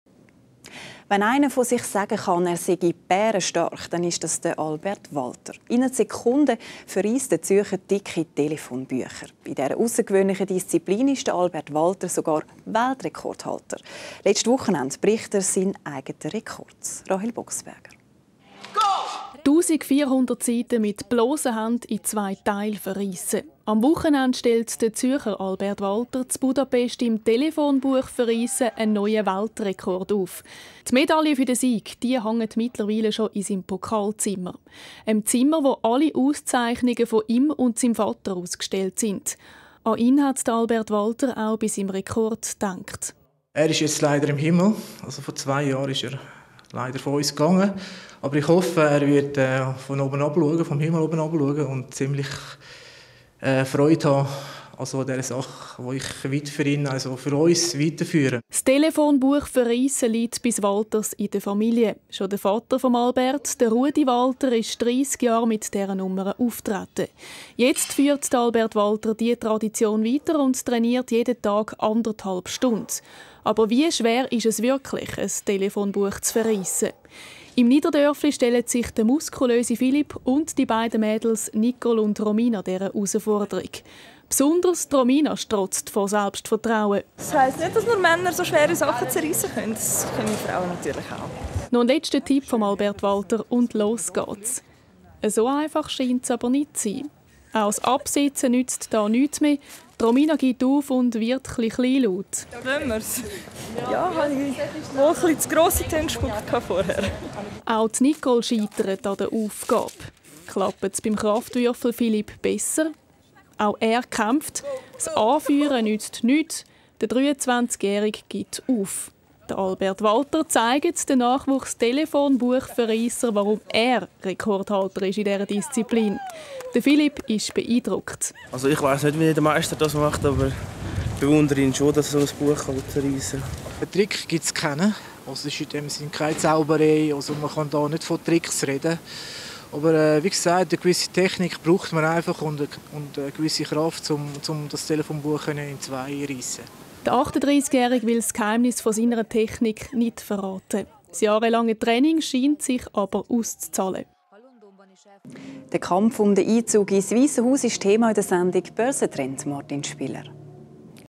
ALBERT vs NEW YORK PHONE BOOK
HUNGARY BUDAPEST * RIPPING NEW YORKER PHONE BOOK
* 1440 PAGE @ 2,7 sec  WR*
ALBERT JAMIE WALTER  FROM ZÜRICH SWITZERLAND
2012/2013